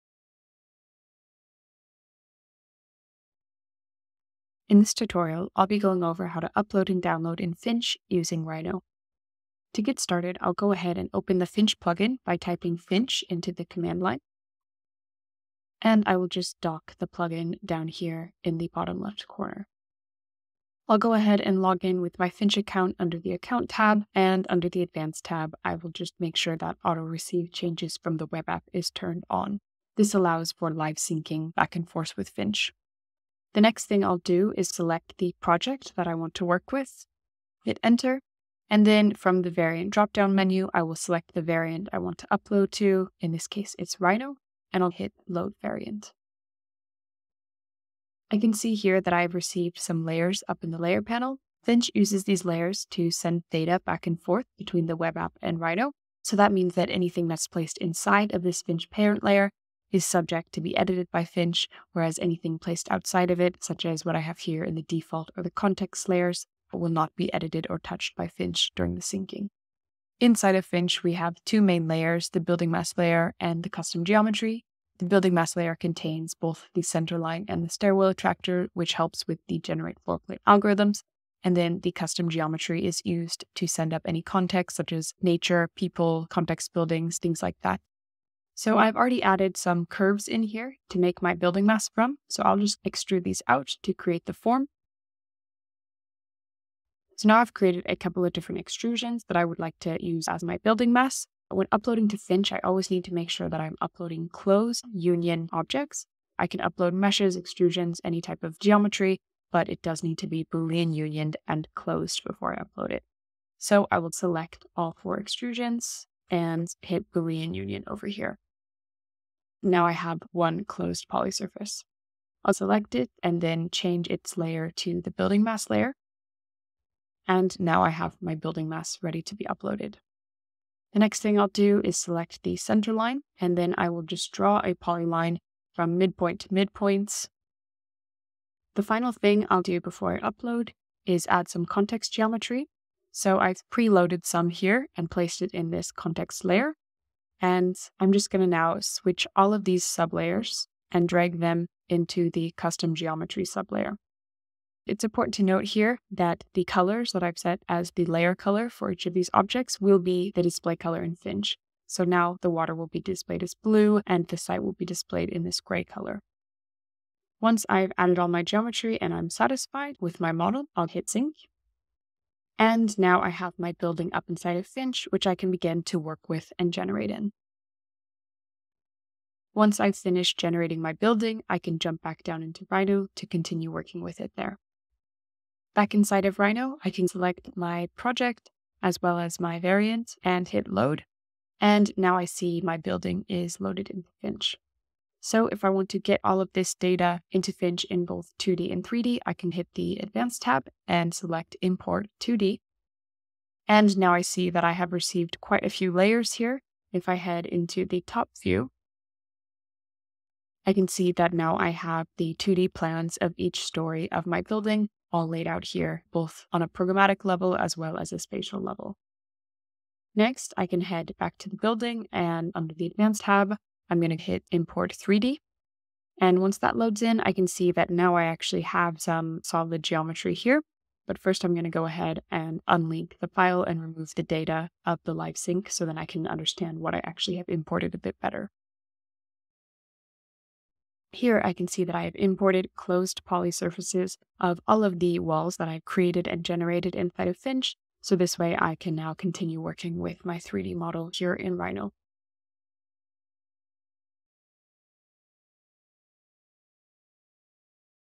Finch Rhino Plug-in
An overview of how to use the Finch plug-in in Rhino to send geometry up to Finch as well as download data from Finch back into Rhino.

The stairwell attractor has been moved into Finch, you no longer need to upload it via Rhino.